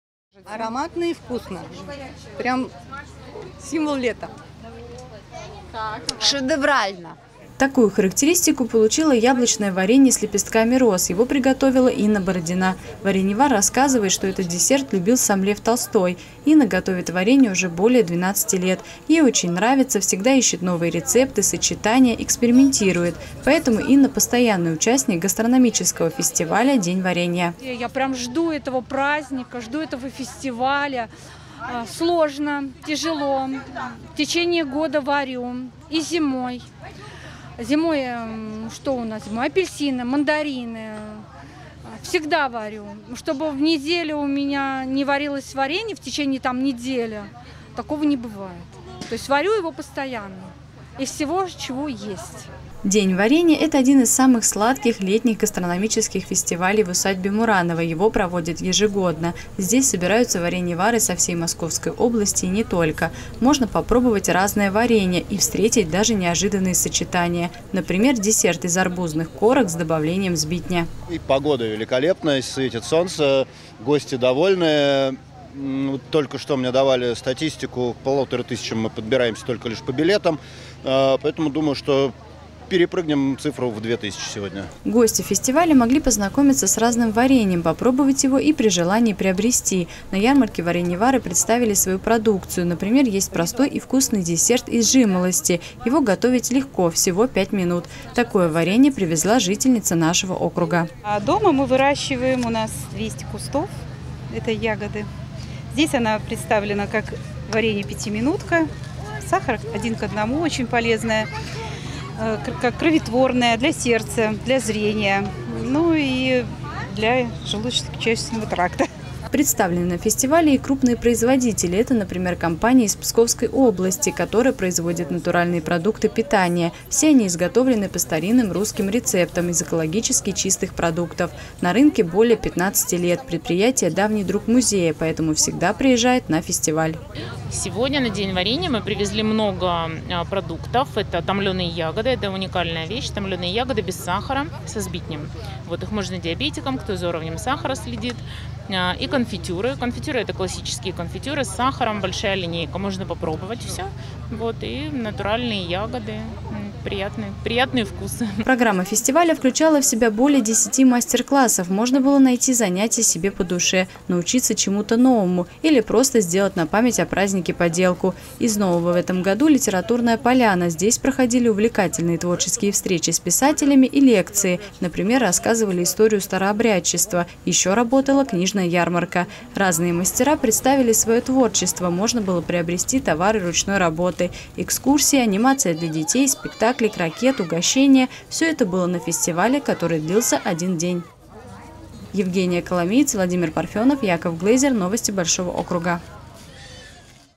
Самый сладкий фестиваль. В усадьбе "Мураново" отметили День варенья
В этом году летний гастрономический фестиваль "День варенья" проходил всего один день. Но программа осталась такой же интересной, разнообразной и насыщенной. Даже добавили новое. Более 1,5 тысяч человек приехали, чтобы попробовать разное варенье, поучаствовать в мастер-классах, послушать лекции и просто замечательно провести время.